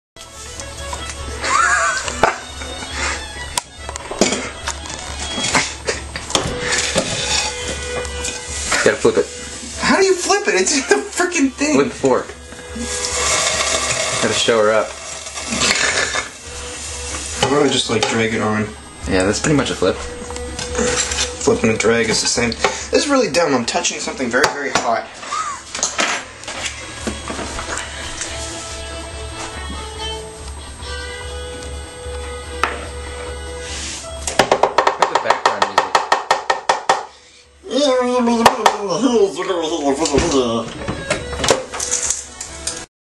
Matthew making waffles
how to pour batter into a waffle iron.